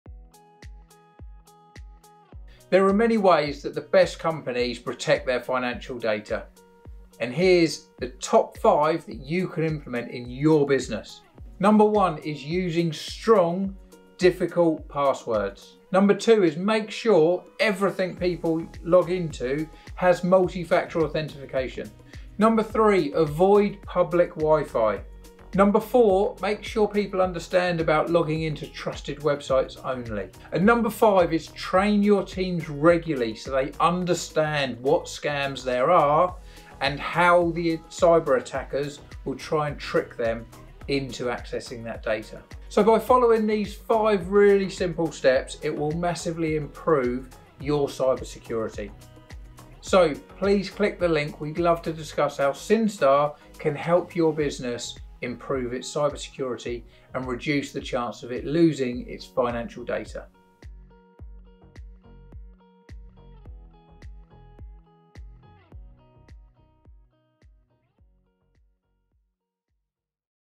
How To Protect Your Financial Information Against Cyber Threats
WARNING! 🚨 Your financial information is at risk of a Cyber Attack! 😱
2.39 million cases of cyber-attacks affected UK businesses over the past 12 months. 🏢📉Don’t fall victim to one.

Statistic: UK Cyber Security & Data Breach Stats 2023 - Databasix UK (dbxuk.com)